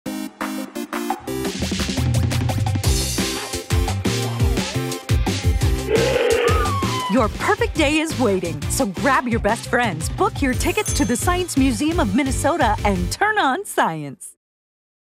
Spring Break 2024 Perfect Day (:15)
Explore, experiment, and expand your mind — no travel required. Make the Science Museum of Minnesota your spring break destination and journey to ancient cultures, launch into deep space, and run, jump, and puzzle your way to STEM fun and learning. Plan your visit today! smm.org